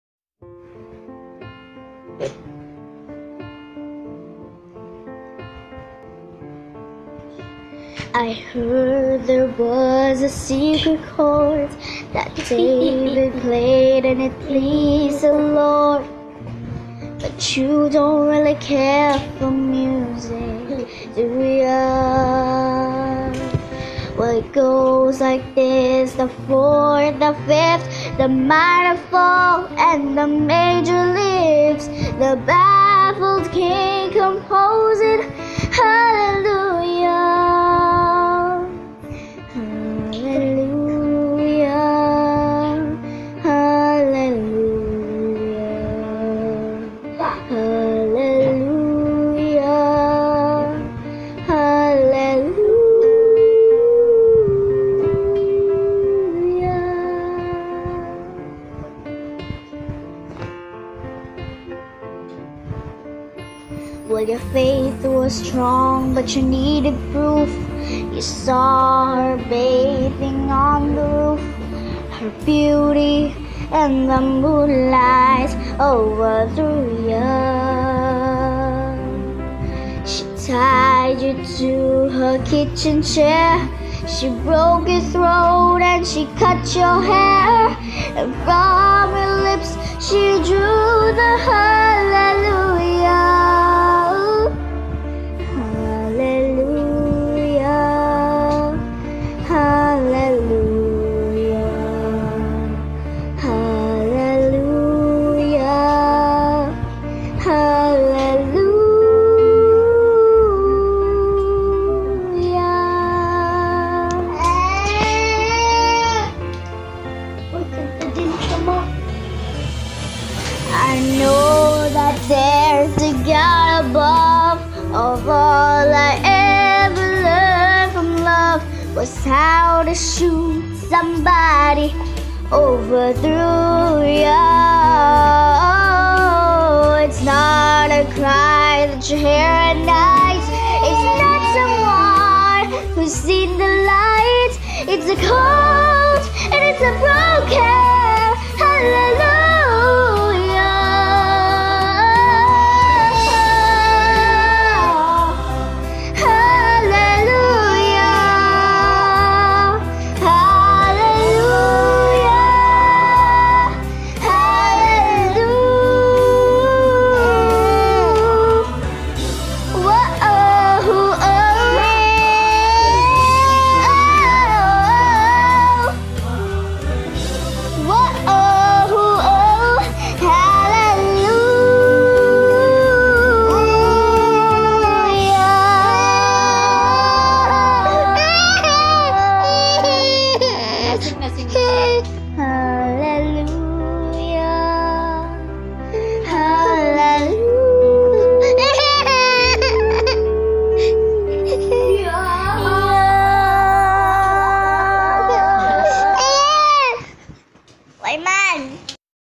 Jem with her baby brother
Lj wants to sing also and He cried because we take away the phone from Him hehehe so cute...